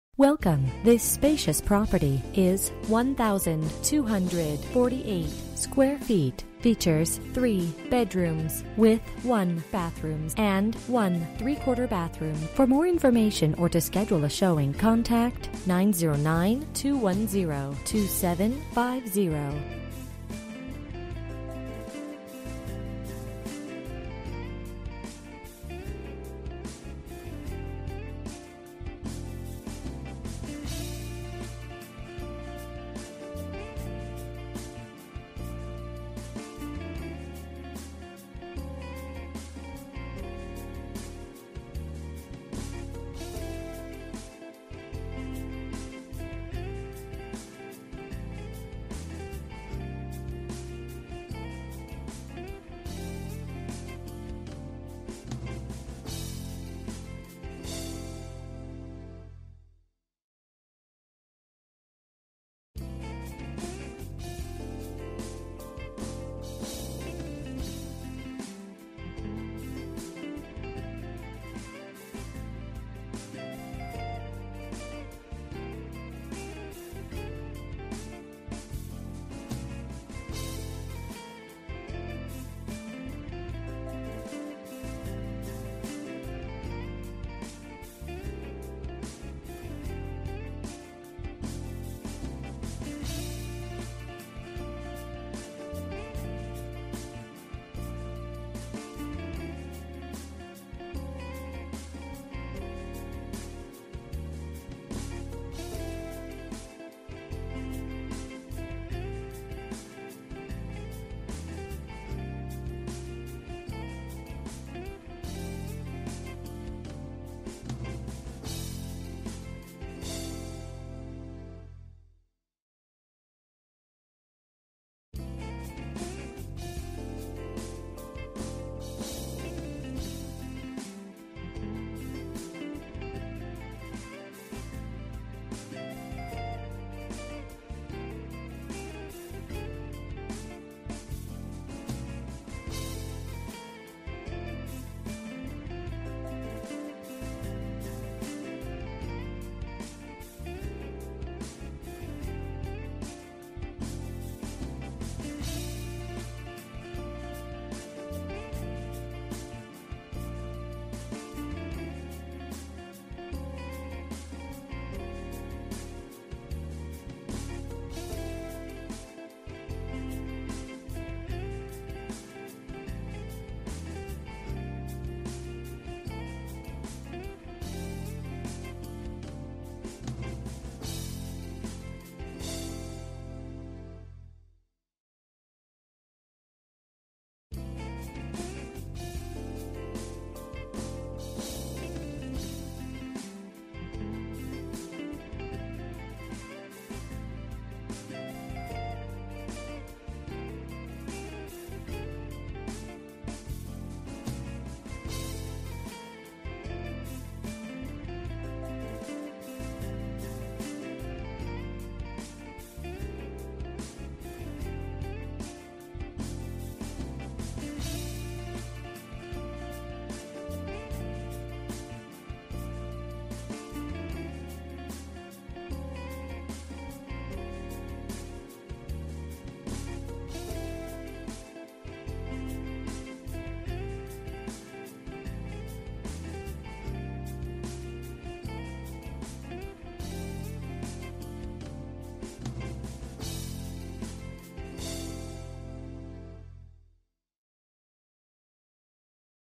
Property for sale - 14335 Jicarilla Road, Apple Valley, CA 92307
Don't miss out on the opportunity to own this lovely ranch style home on an approx 1-acre lot.  Plenty of space and privacy here, and fully fenced and cross fenced.  Home features 3 nice-sized bedrooms, including the master suite with its own attached bath and sliding glass door out to back patio area.  Spacious family room area with cozy wood burning fireplace for those cold winter nights.  Galley-style kitchen with electric range, hook-ups for clothes washer and a built-in ironing board.  Light and bright nook just off kitchen perfect for family dinners.   Breezeway entrance to both the house and the semi-detached 2-car garage.  Dryer hook-ups in garage.  Newer evaporative cooler and plumbing. Plenty of parking in the driveway and offers two front gates for circle drive-thru parking.  The backyard is huge and is cross-fenced from the back area which would be perfect for parking large equipment, horses or RVs, and even has a convenient gated entry from the back of the property.  Hurry, this one won't last!  The potential is endless here!

Bedrooms: 3
Bathrooms: 1 full and 1 three-quarter
Square Feet: 1248
Price: $205,000

MLS ID: IV18280445

Juliehudsonhomes.com